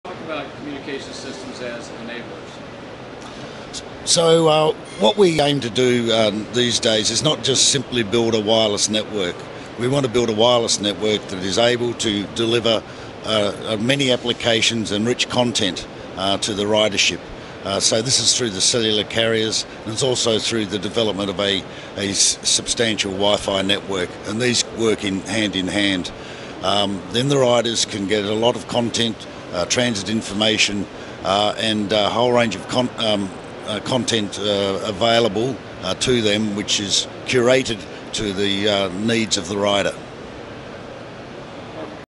MWC14 SOLiD & BAI Canada: Communications Systems as Enablers
SOLiD Technologies and BAI Canada don't just want to build wireless networks, they want to build ones that are capable of delivering a wide variety of applications and media rich content. Chris Jaeger, President of BAI Canada, explains how.

MAQ00271 solid 7